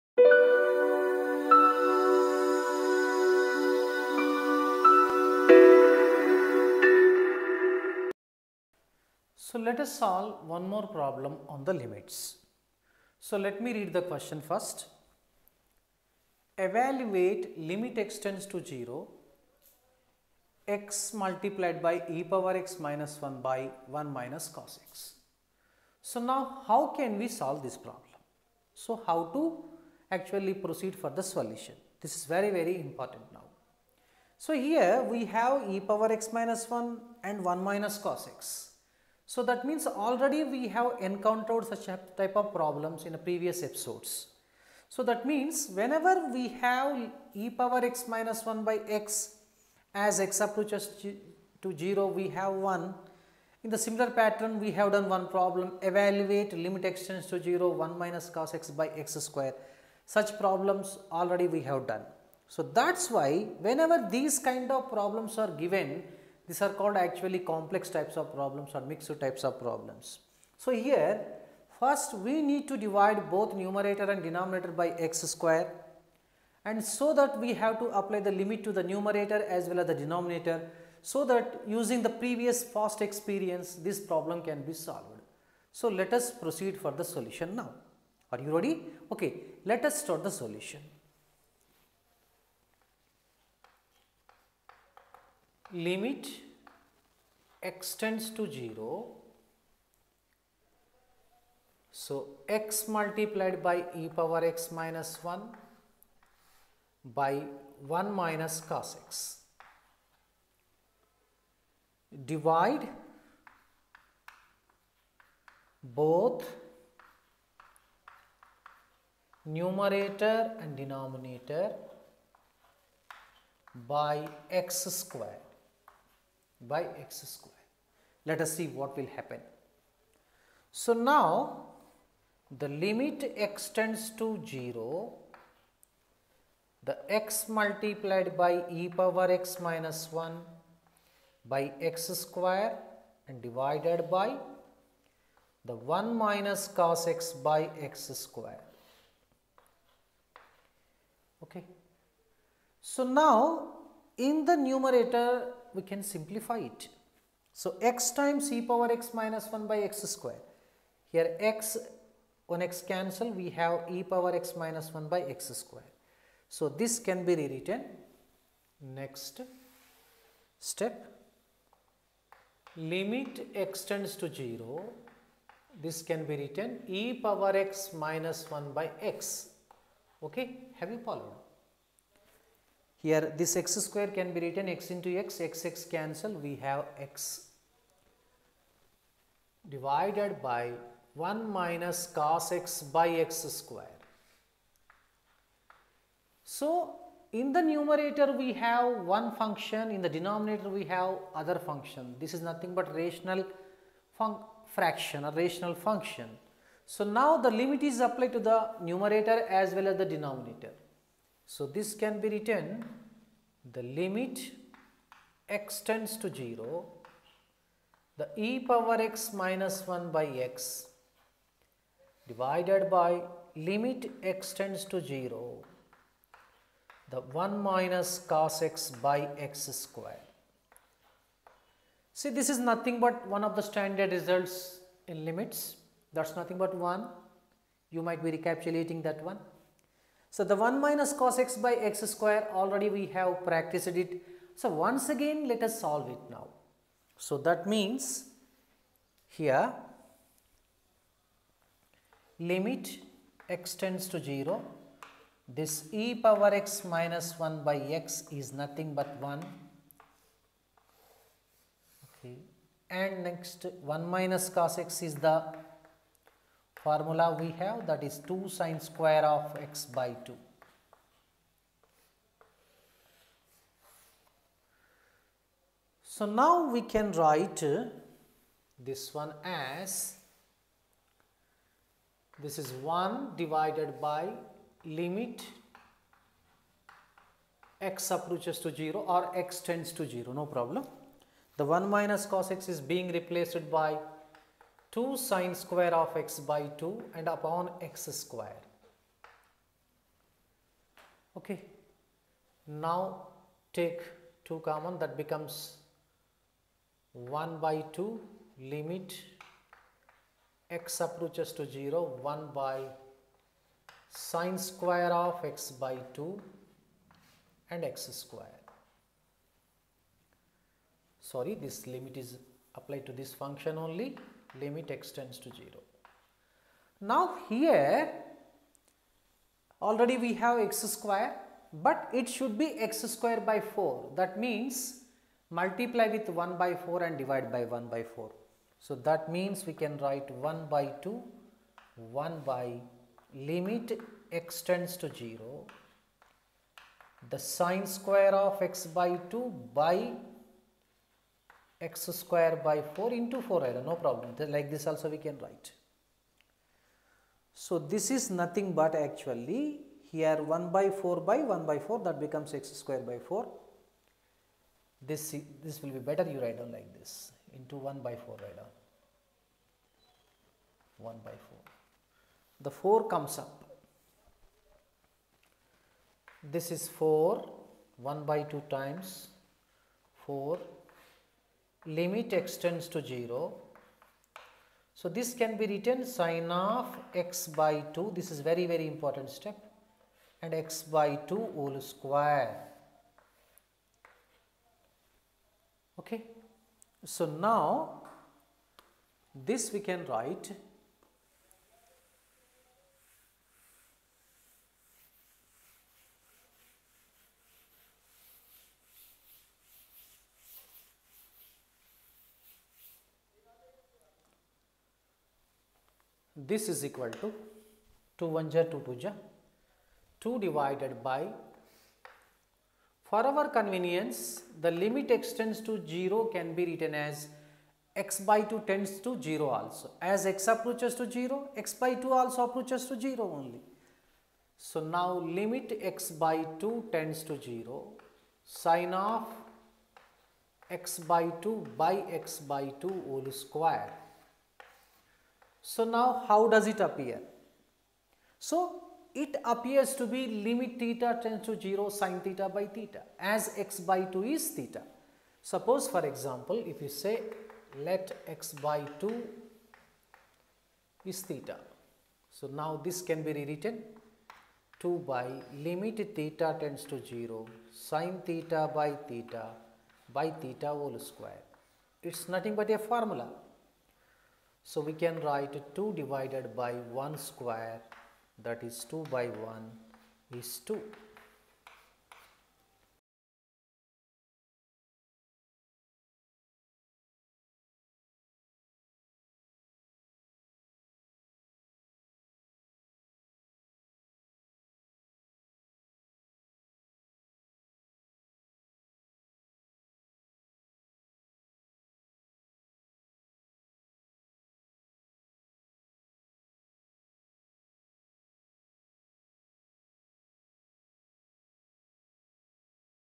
2 marks || Limits and Continuity Part - 18
After watching these free videos the learner will be able to:

1) Know the concepts of Limits
2) Evaluate the limits using the substitution method
3) Apply the standard formulae on the Limits
4) Solve the different problems using the standard results
5) Solve the problems on left hand limit & right hand limits
6) Interpret whether function is continuous & Dis-continuous
7) Solve the problems on continuity
8) Apply the  concepts while formulating the basic results in derivatives